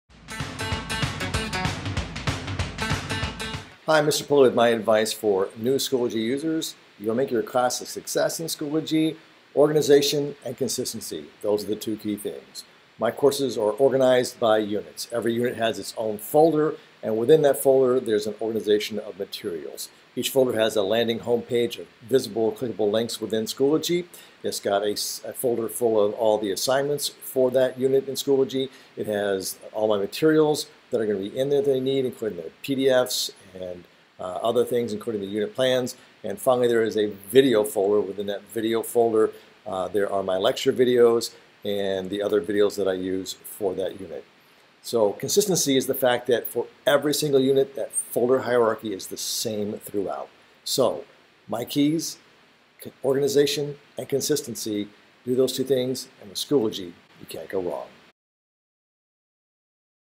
Schoology New User Advice
My idea of what is important for new users of Schoology? Organization and Consistency.